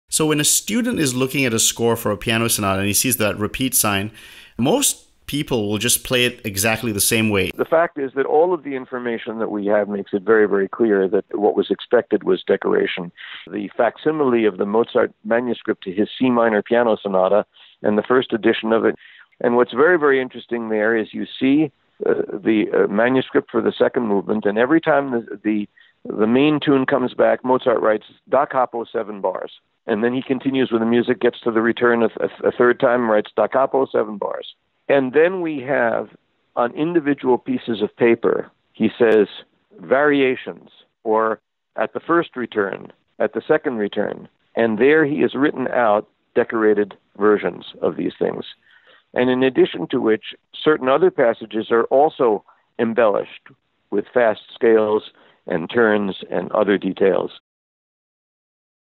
Embellishments and variations on repeats
Acclaimed Classical Pianist, Composer, Improviser, Musicologist and Conductor, Robert Levin talks to Nikhil about how Mozart’s manuscripts strongly indicate instructions to create variations and embellishments on repeats!